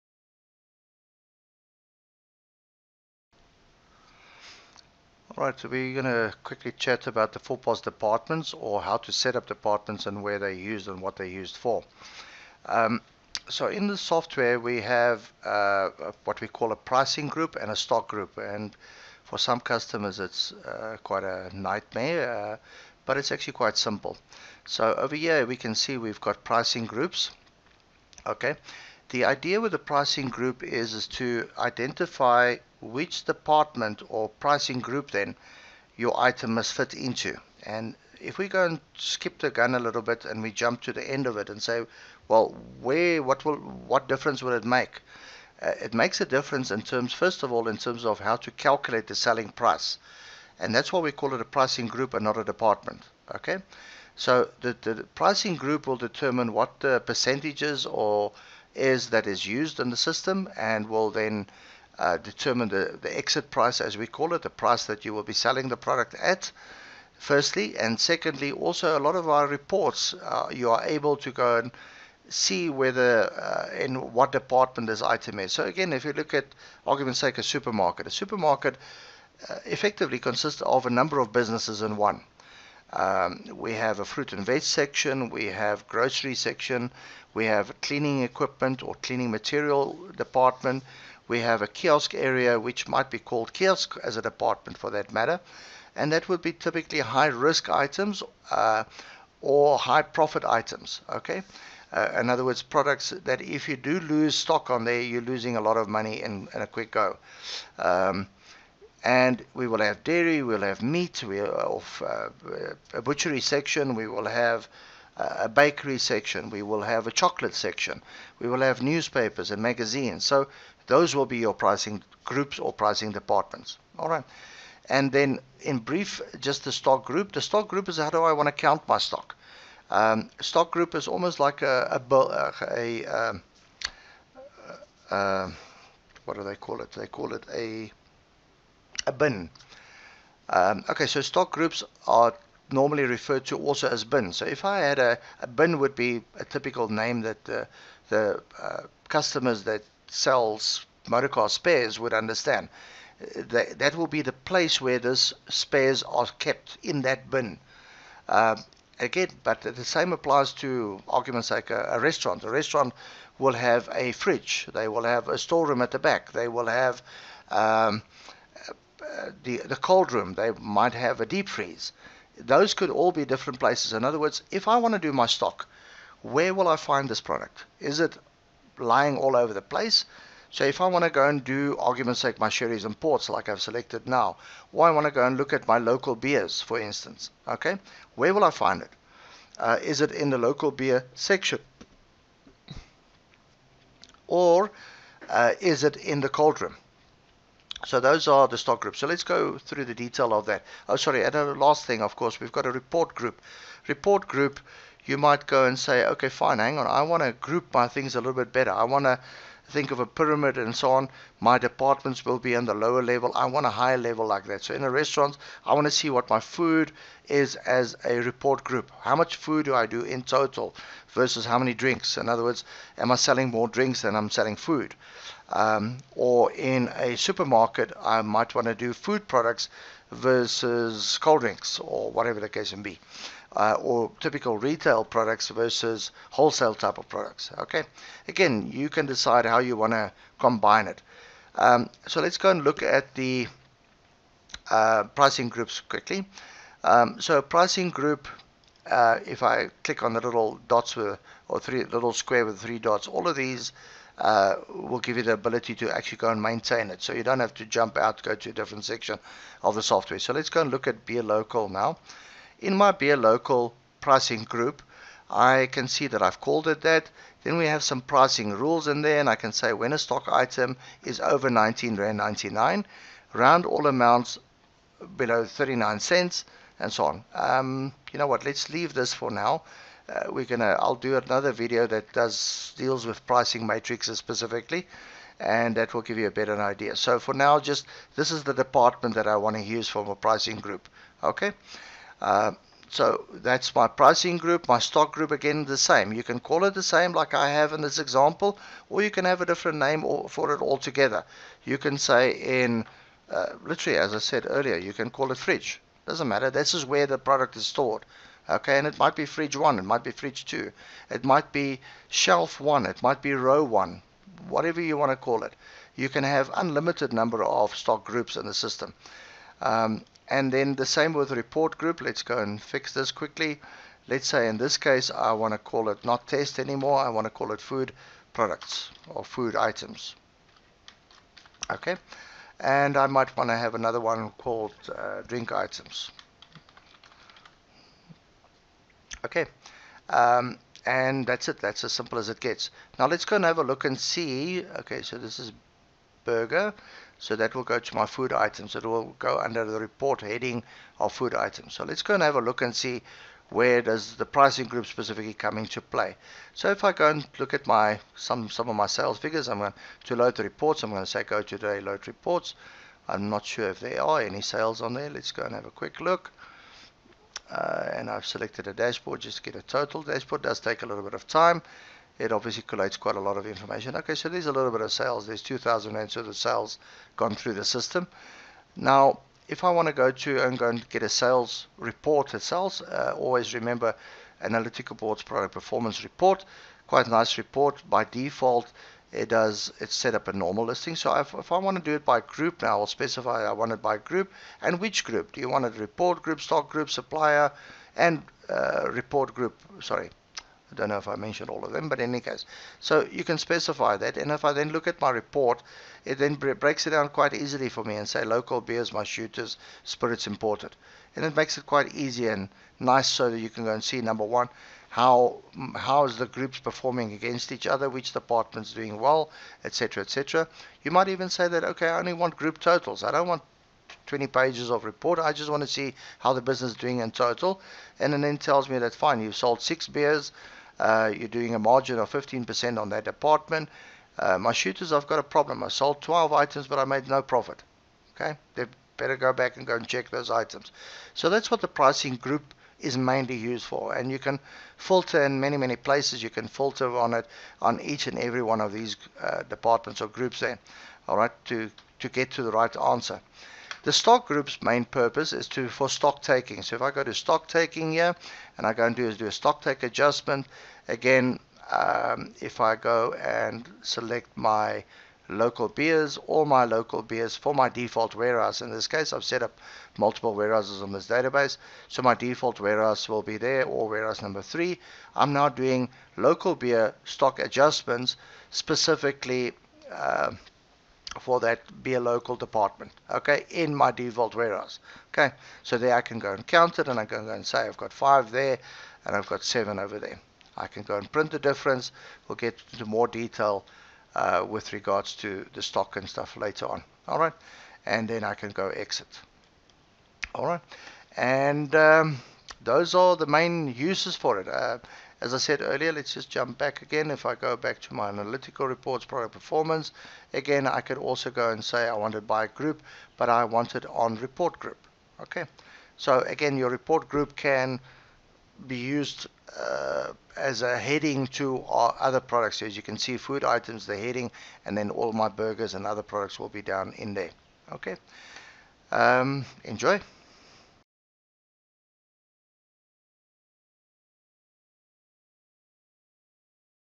4POS - How to setup departments in 4POS
4POS Departments setup, how they used and what they are used for.

Pricing Group is used for calculating the selling price for instance.

Stock group is almost like a "bin" - how do i want to count my stock when i do stock take. Great feature that assists with your pricing and stock control in your business.

0:00 Introduction
0:25 Setting up your Pricing Groups
2:10 Setting up your Stock Groups
3:30 Setting up your Report Groups
6:25 Seeing the benefits